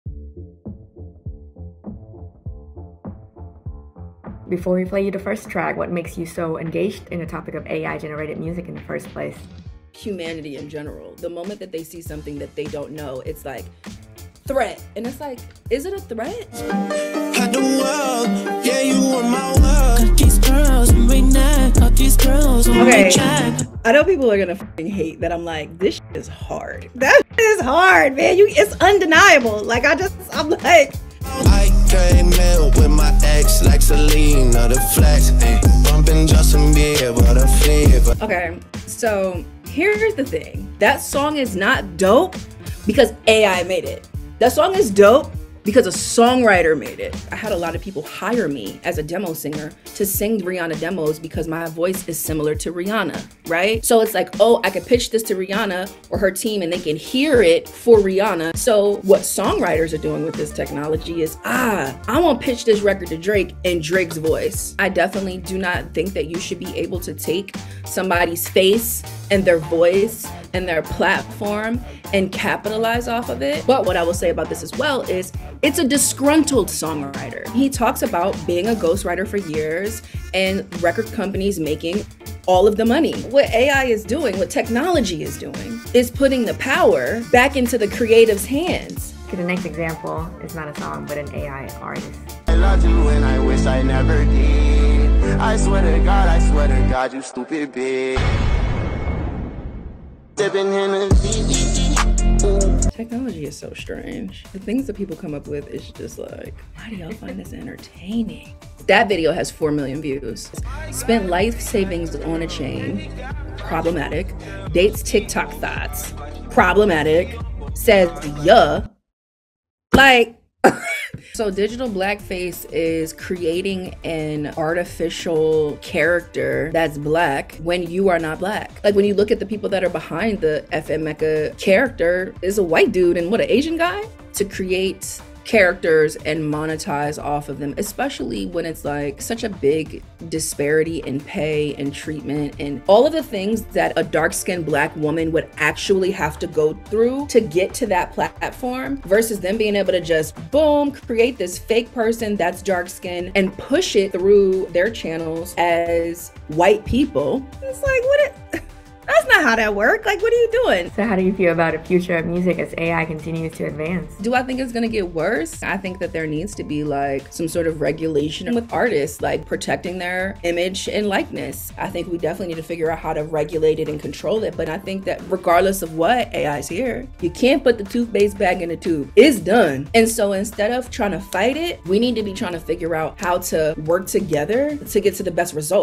Grammy-Winning Artist Takes on AI and the Future of Music
Tiffany Red is a Grammy-winning songwriter who's written hits for Zendaya, Jason Derulo, and Cassie — watch her react to AI-generated music.